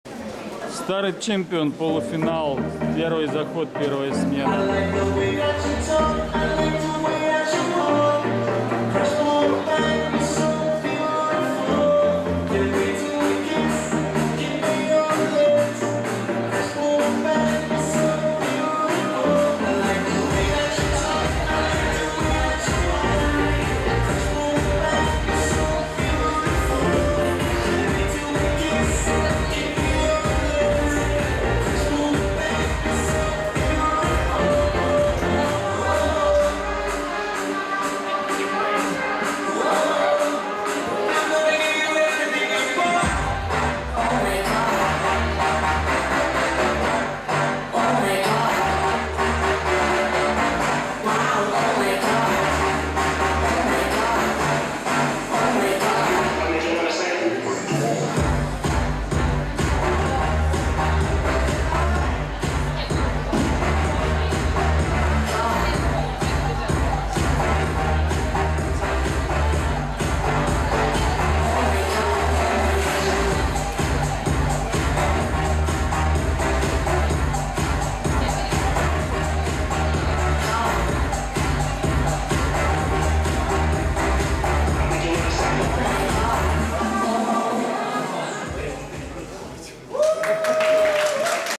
22.10.16 ОКД DnD Star&Champion 1/2 заход 1 танец 1
Осенний Кубок Движения 2016
22 октября 2016 г.
Рейтинговый турнир АСХ
Место проведения: ТДЦ Барон Холл
Организаторы проведения: Ассоциация Спортивного Хастла
Непосредственные организаторы: Школа танцев Движение, Никита Гриненко и Ко
видеооператор: Игорь Скобелев